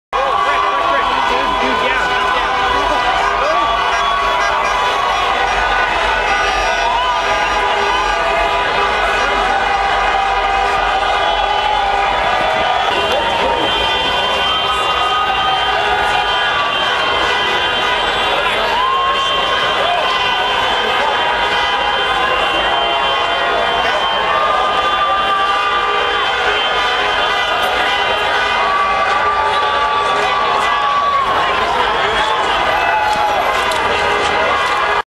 Officer down on Yonge street
Walking North on Yonge street this officer was down and looked seriously injured.